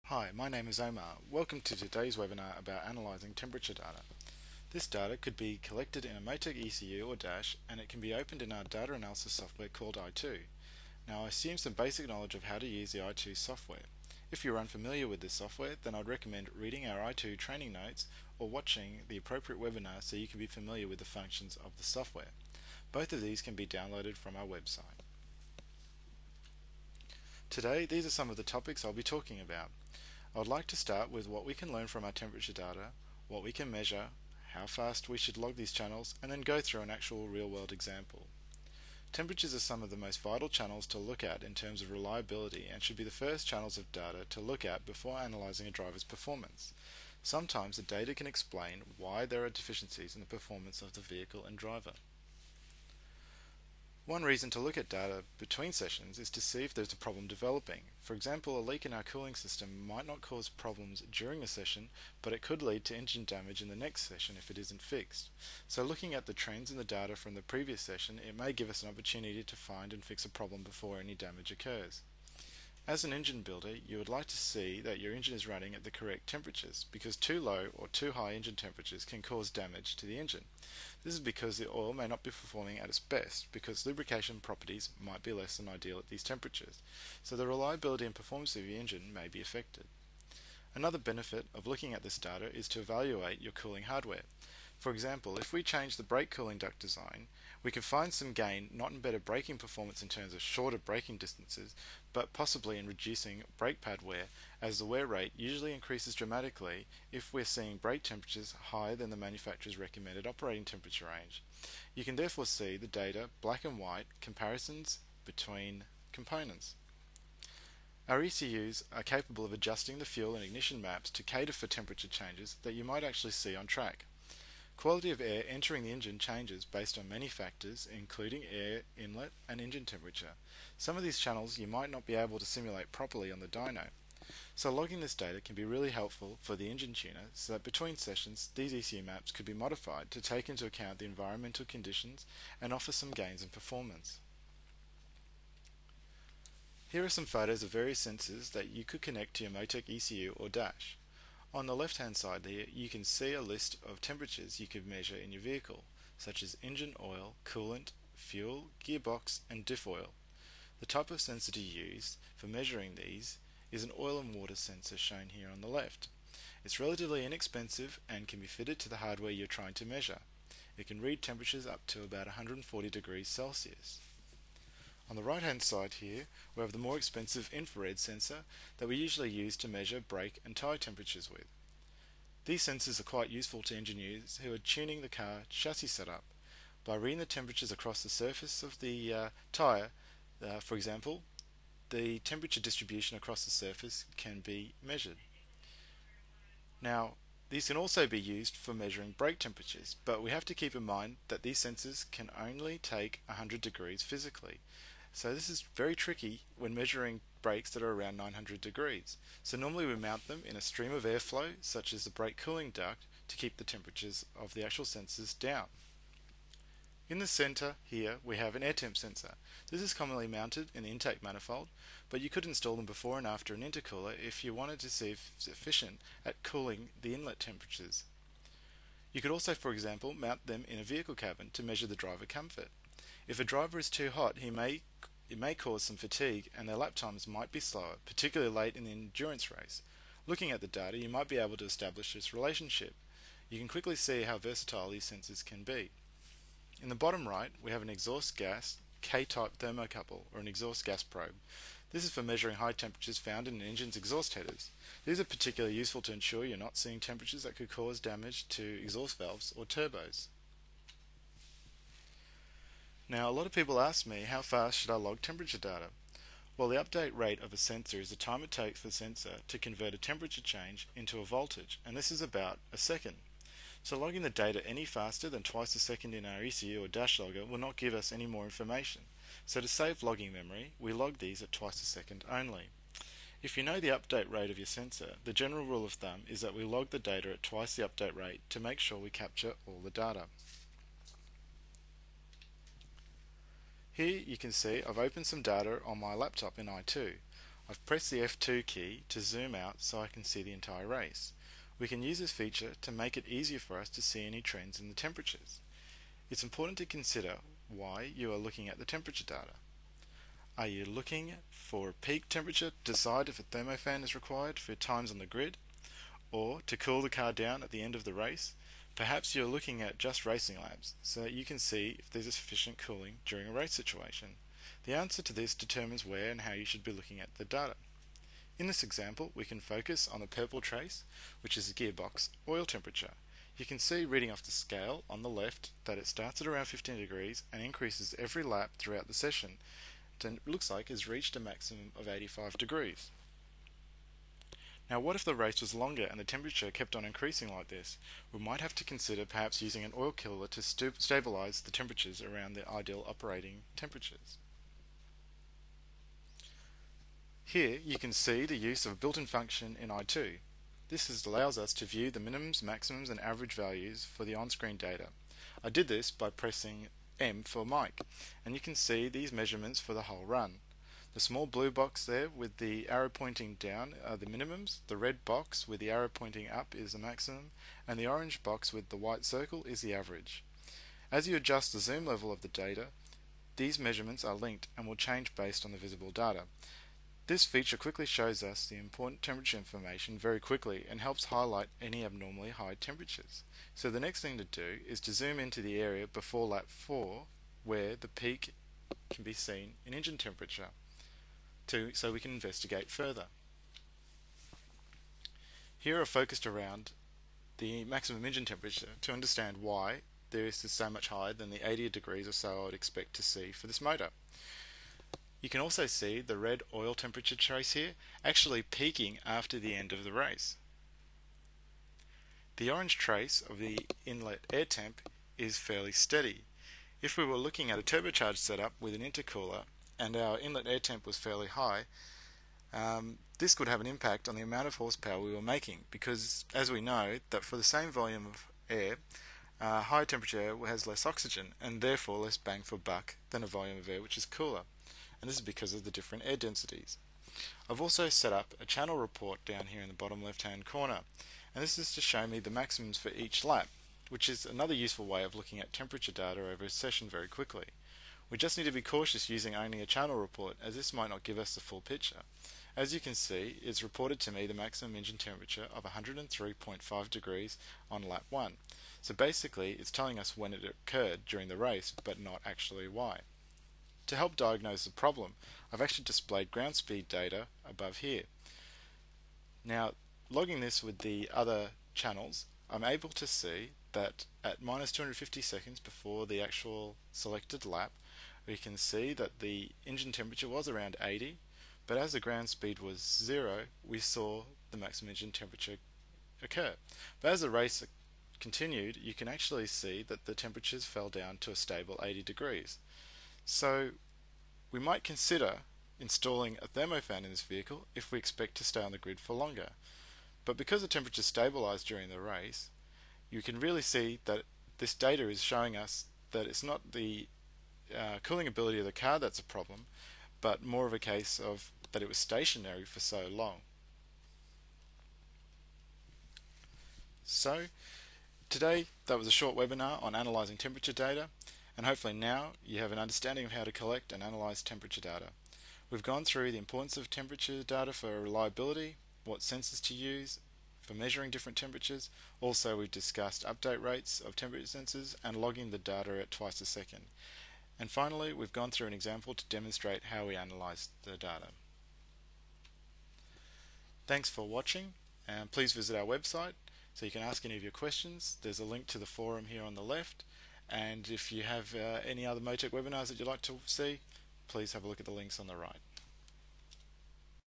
MoTeC i2 Training: How to analyse temperature data
Temperatures are some of the most vital channels to look at in terms of reliability and should be the first channels of data to look at before analysing a driver's performance. Sometimes this data can explain why there are any deficiencies in performance of the vehicle and driver.